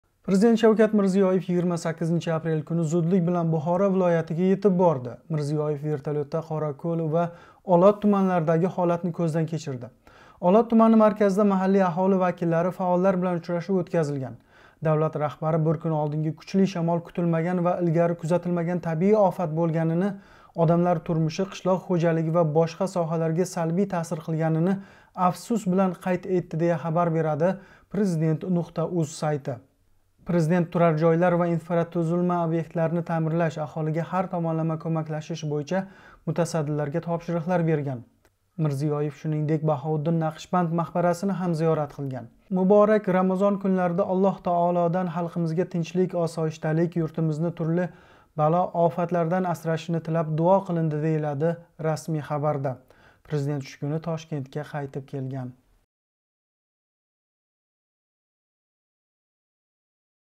Президент Мирзиёев кучли шамолдан зарар кўрган Бухорони бориб кўрди - BBC Uzbek
Президент Мирзиёев кучли шамолдан зарар кўрган Бухорони бориб кўрди

Мирзиёев вертолётда Олот ва Қўракўл туманларидаги зарарни кўрган ва мутаддиларга топшириқлар берган.

Давлат раҳбари шунингдек, маҳаллий аҳоли вакиллари билан учрашган.